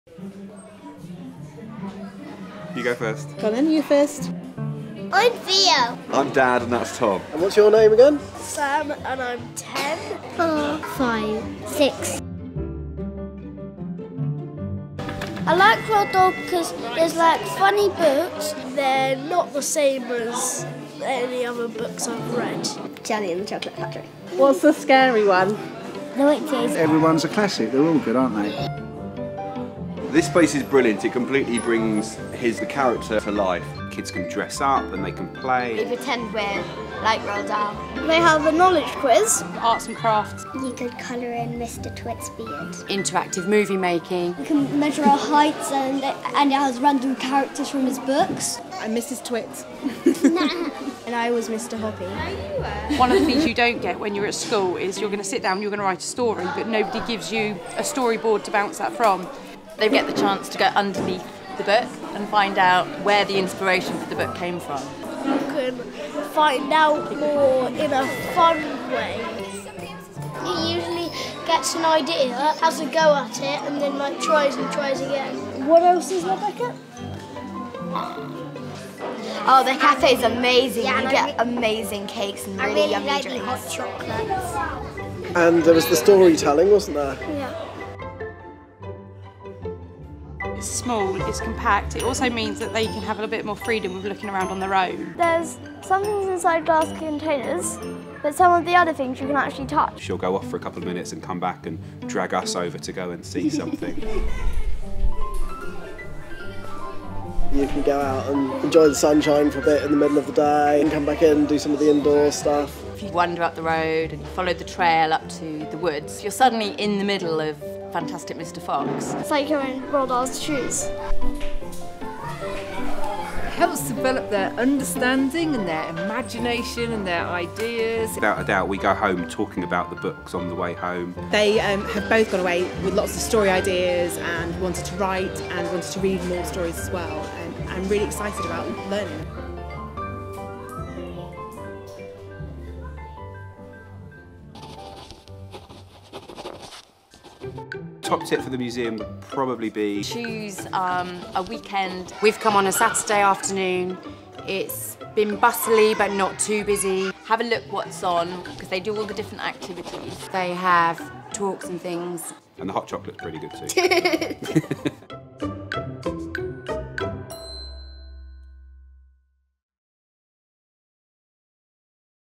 The Roald Dahl Museum and Story Centre
This great little Museum is aimed at 6-12 year-olds and their families and is situated in Roald Dahl’s home village. Visitors can find out about Roald Dahl’s life and stories using touch-screen monitors, hands-on exhibits and activities. Workshops, events and free storytelling take place at weekends and in school holidays.